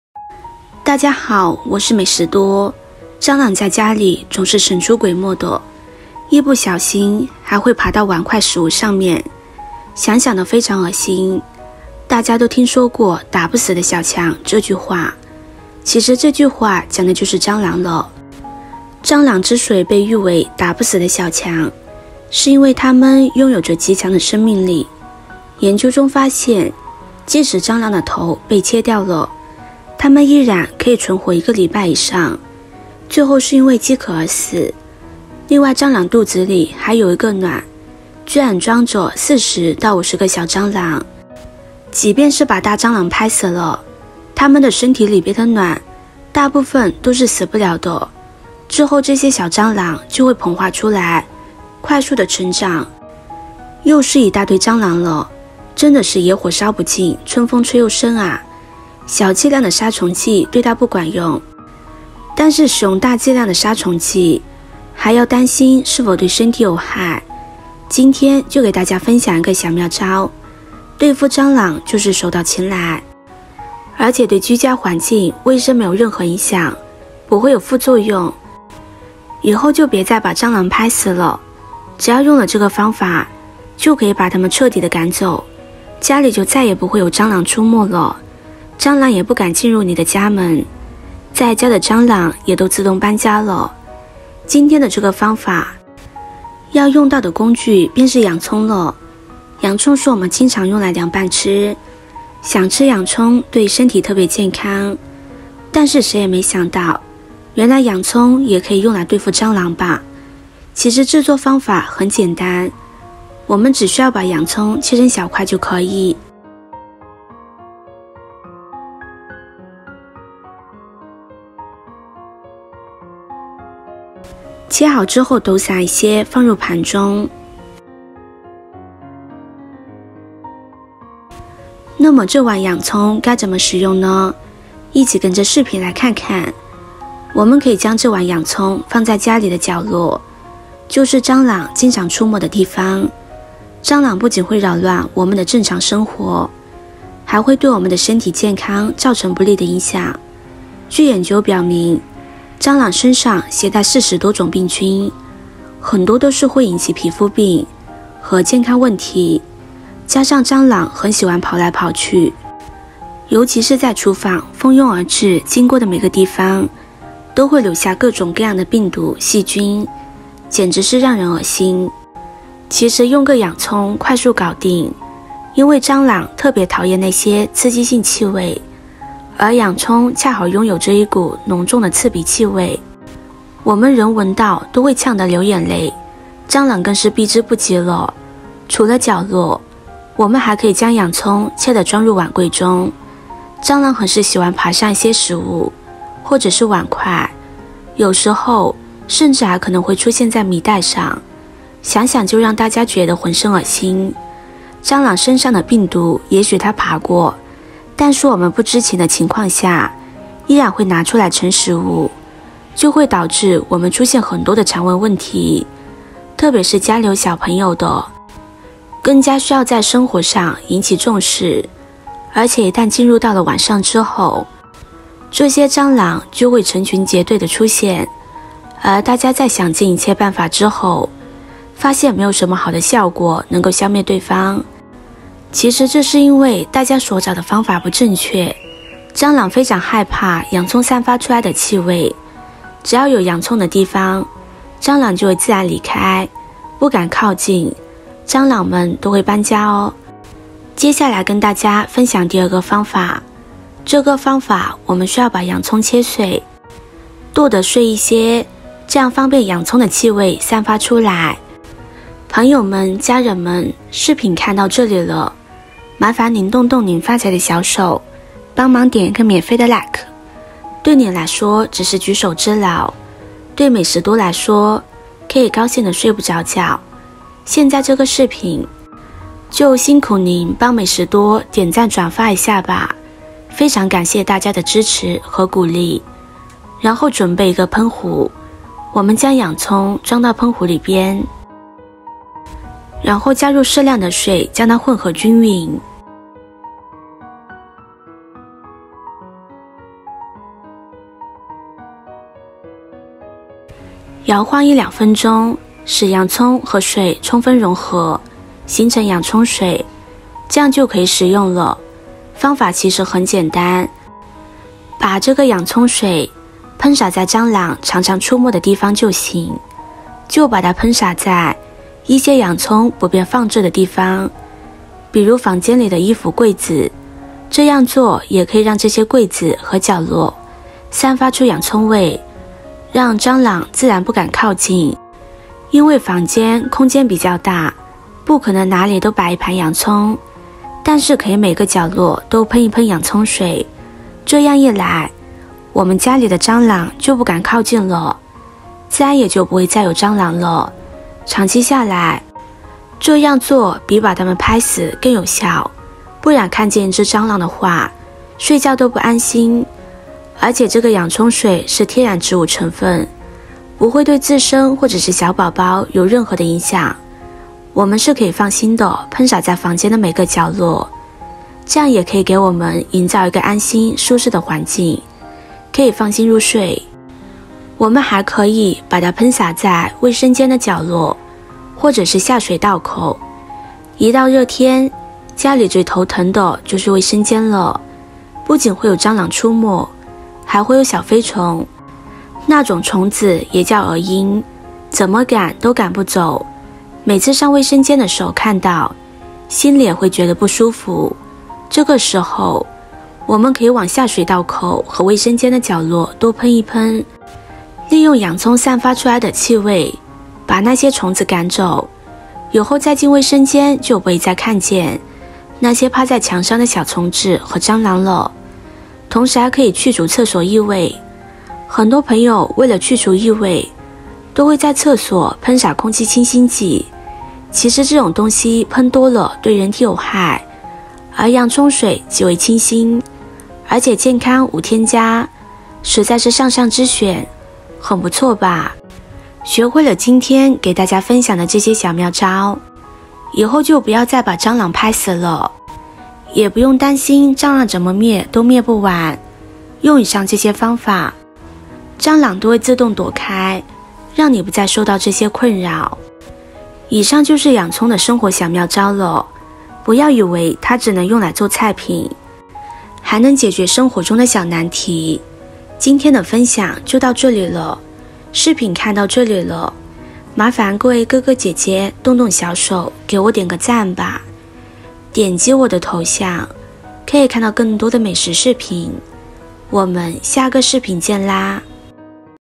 蟑螂原來最怕它，把它放在家中，比蟑螂藥還管用，蟑螂來多少滅多少，【廚房美食多】It turns out that cockroaches are most afraid of it.
蟑螂原來最怕它，把它放在家中，比蟑螂藥還管用，蟑螂來多少滅多少，【廚房美食多】It turns out that cockroaches are most afraid of it. Putting it in your home is more effective than cockroach medicine. It will kill as many cockroaches as they come.

每天都會為您帶來一道 家常菜 🍲
給美食多的影片來一個 👍🏼
有最新影片，就會馬上通知您

美食教程